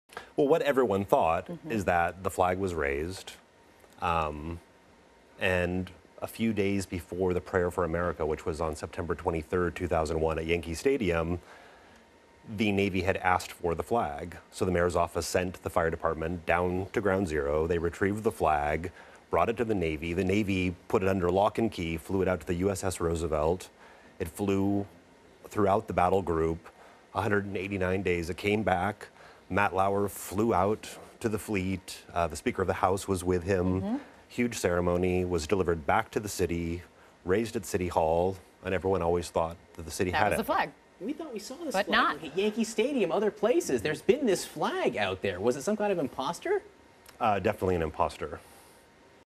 DIR OF DOCUMENTARY 'THE FLAG':WHERE FLAG WENT
A new CNN documentary focuses on what happened to the flag raised among the WTC rubble.